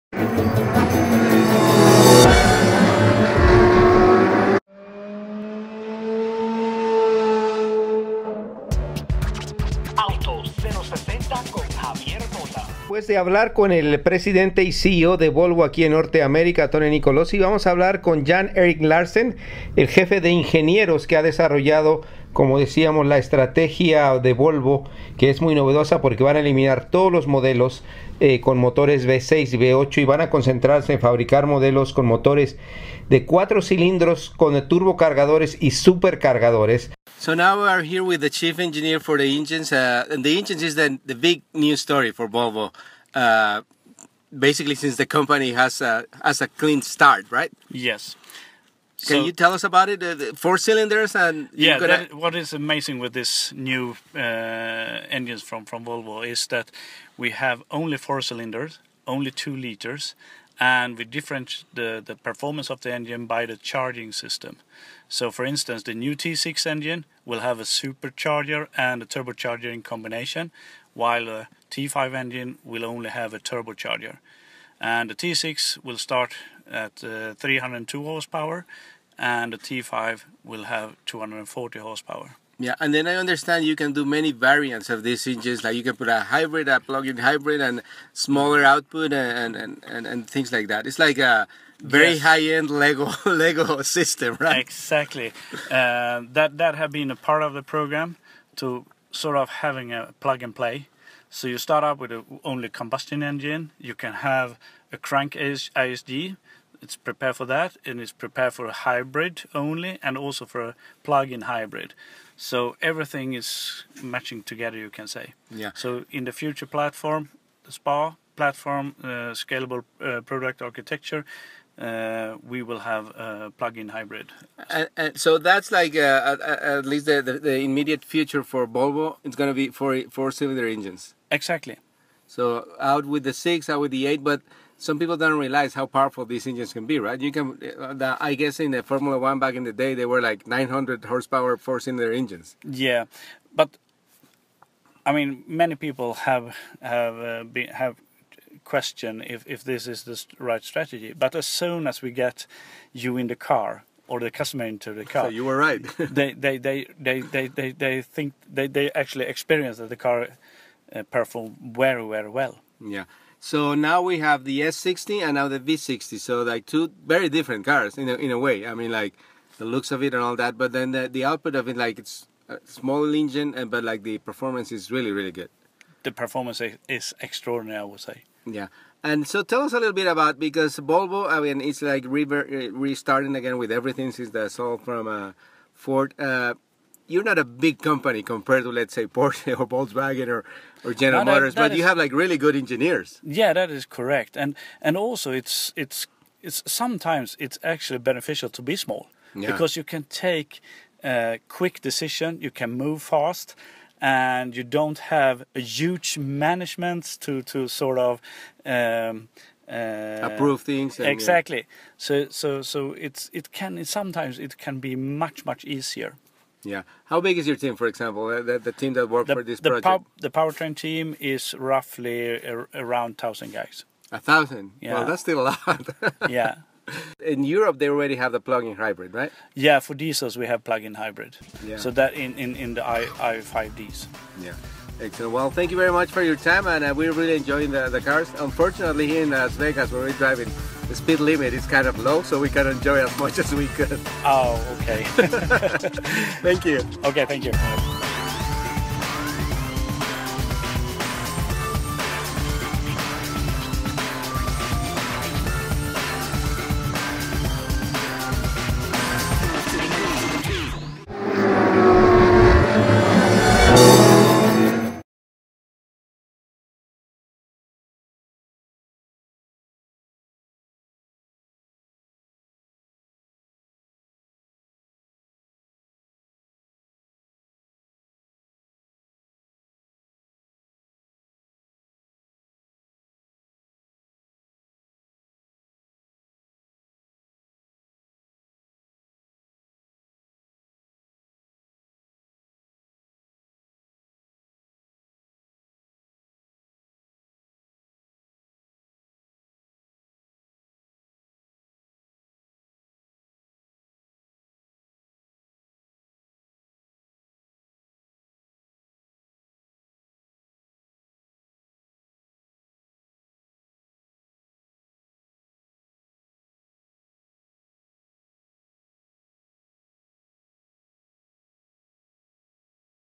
Autos 0-60 - Jan Erik Larosson Volvo, Drive E Chief Engenieer
An interview with Jan-Erik Larosson Volvo's Drive E Chief Engenieer during the first drive of the 2014 Volvo S60 and Volvo V60 in Las Vegas.